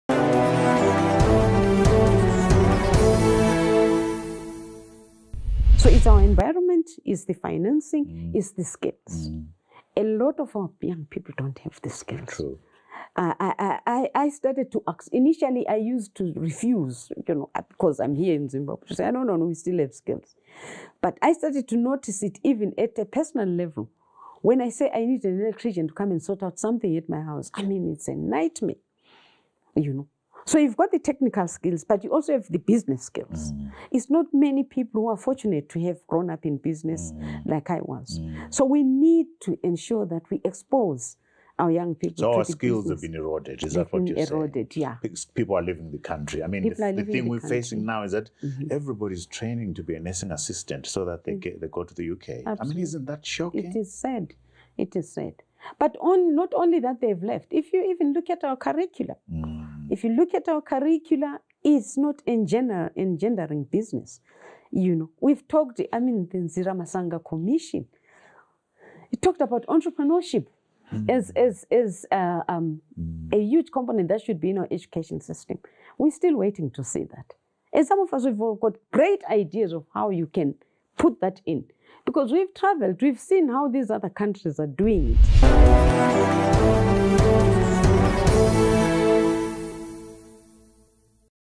Most Young People in Zimbabwe Don't Have Skills - Sibusisiwe (Busi) Bango
Most young people in Zimbabwe don't have skills - They lack exposure to the business says Sibusisiwe (Busi) Bango in this week’s quality conversation.

Video produced by AMH, Heart & Soul.